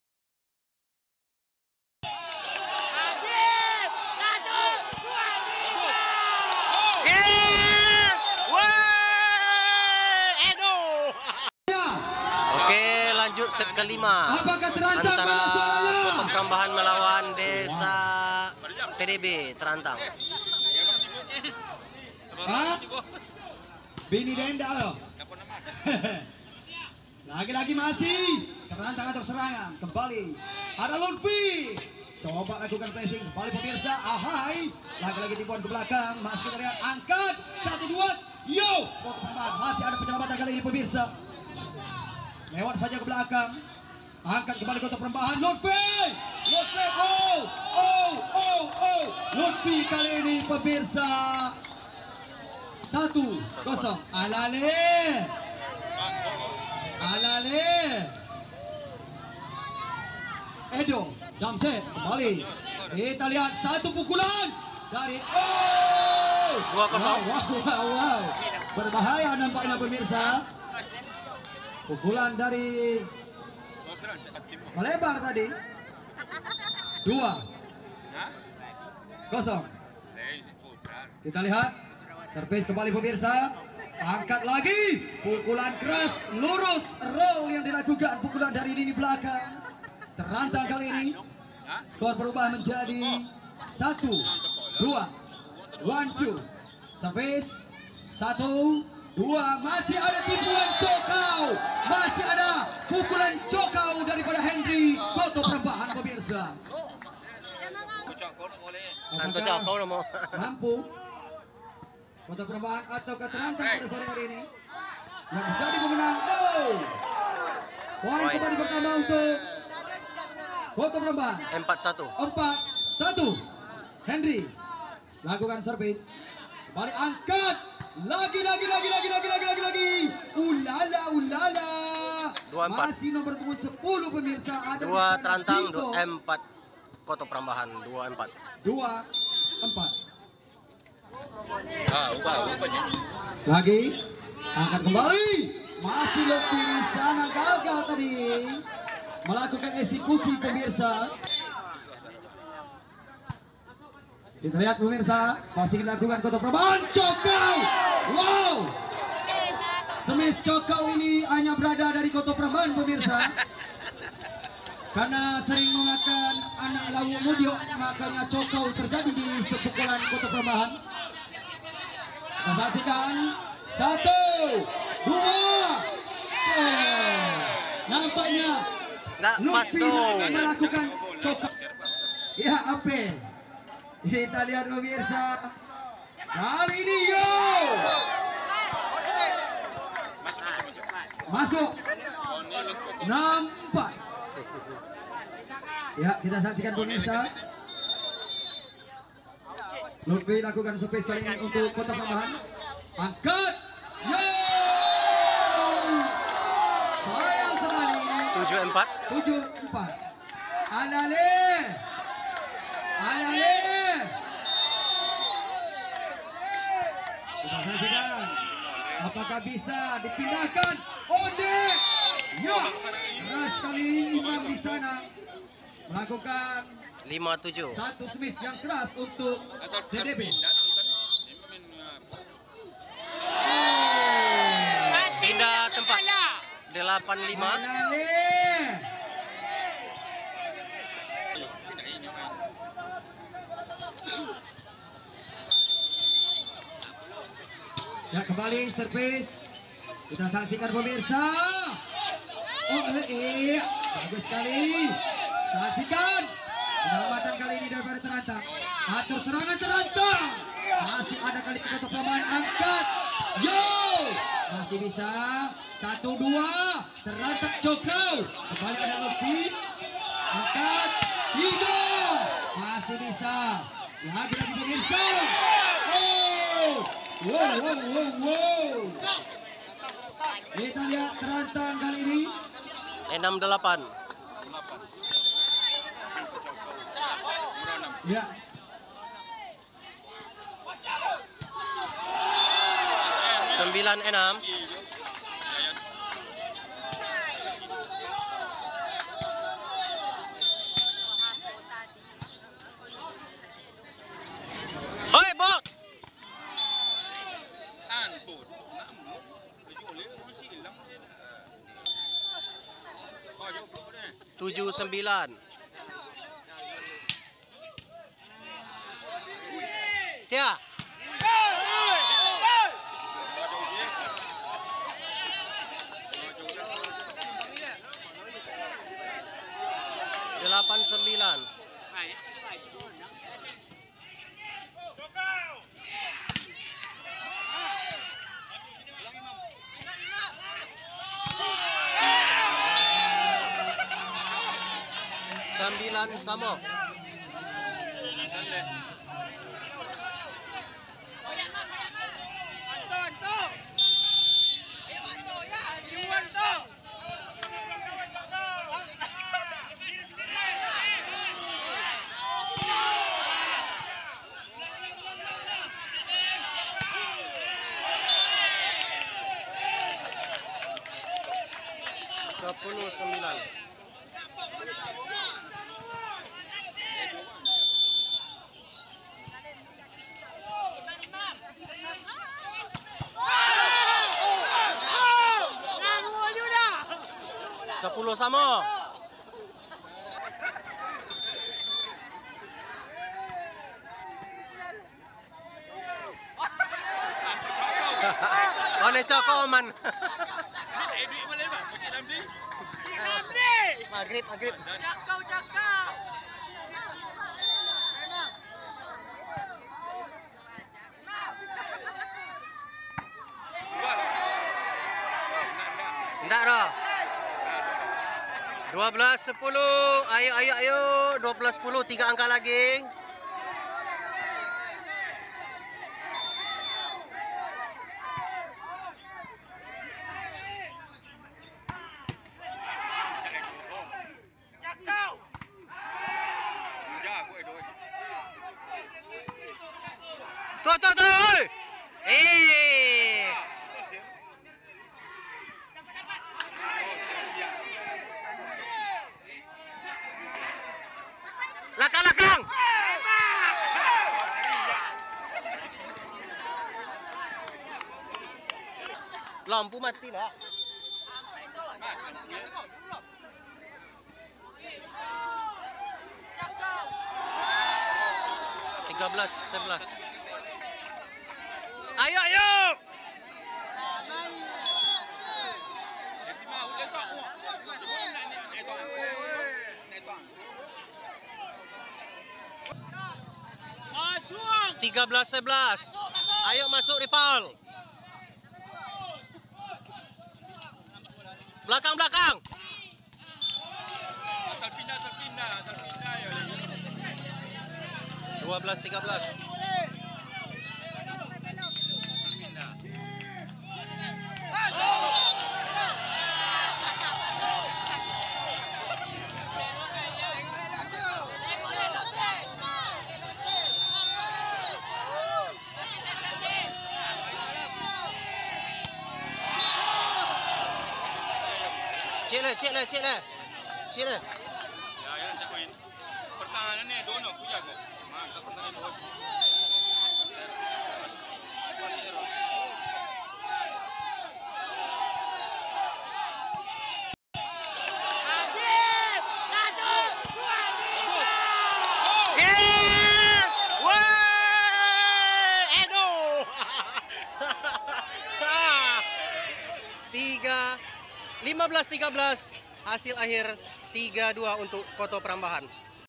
Set V EDO YANUAR SEMAKIN GACOR RAIH KEMENANGAN 3 - 2 ATAS TERANTANG TDB.
Perempat Final Semi Open Turnamen Bola Voli Rajawali Cup Desa Pulau Birandang 2024.
TERANTANG TDB vs Koto Perambahan